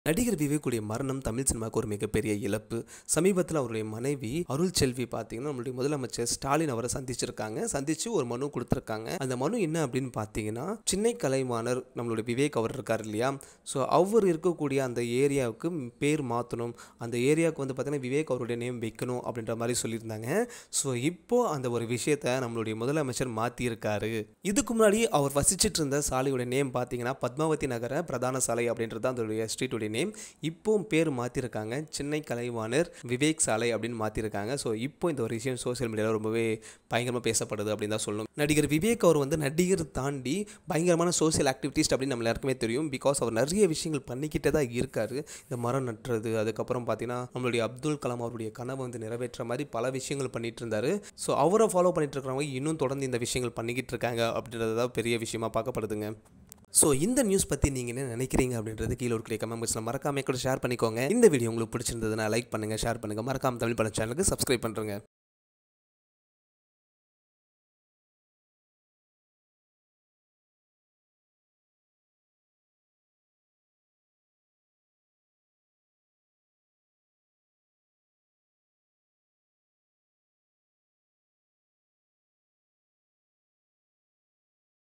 TN Govt. Renames Padmavathi Nagar Road To Chinna Kalaivanar Vivek Road | Tribute To Late Actor Vivek
TN Govt. Renames Padmavathi Nagar Road To Chinna Kalaivanar Vivek Road. It has initiated by the TN government in the memory of Late Actor Vivek.